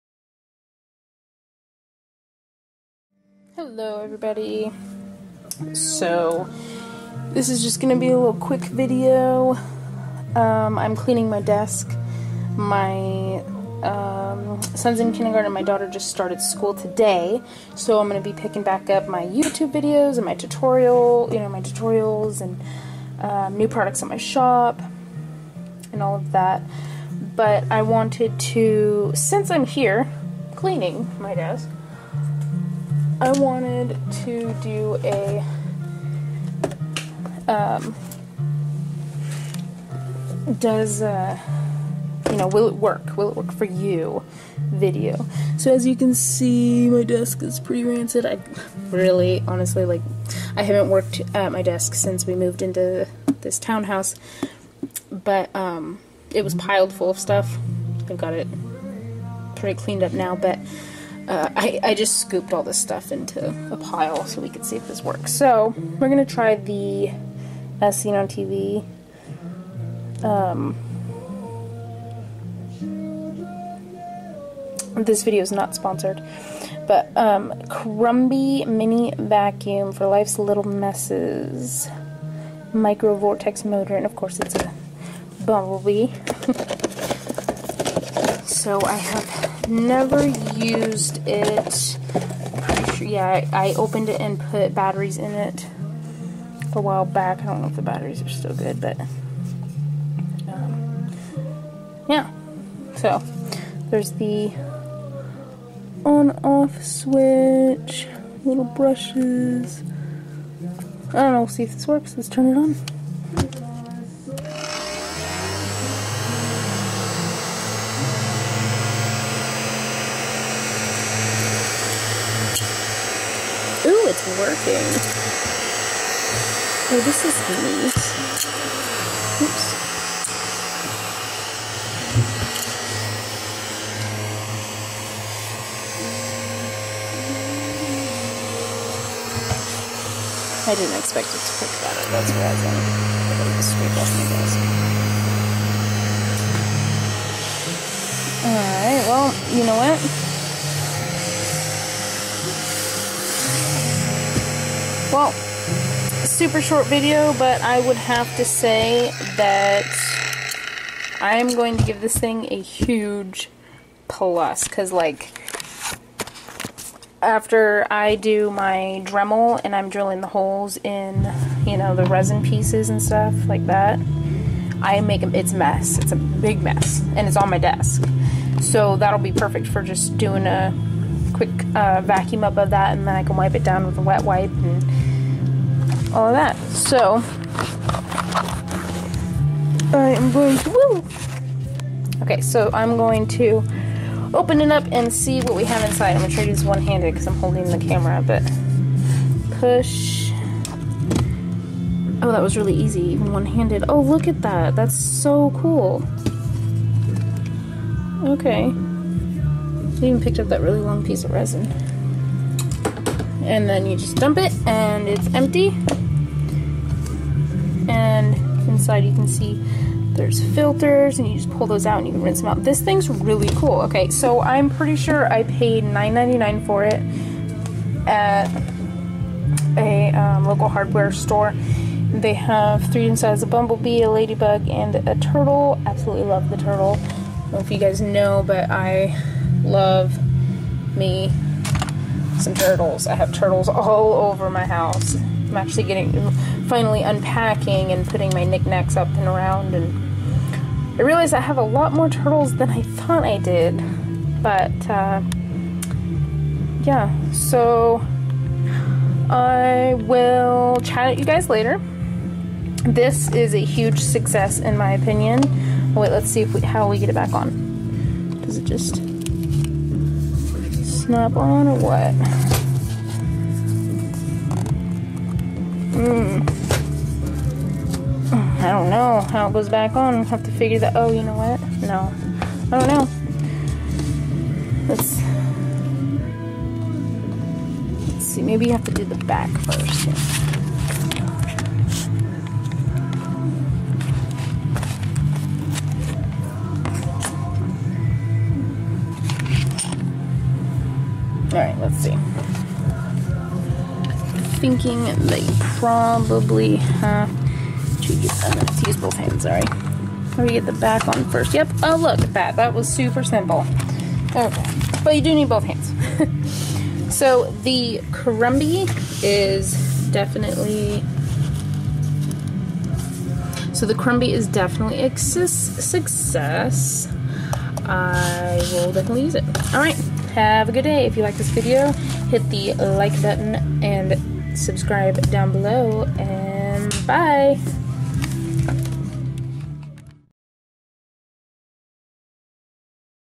Will It Work Wednesday! Mini Bee Vacuum
Join me in testing out this mini bee vac on my desk! Short vid but its a very cute (and useful) :D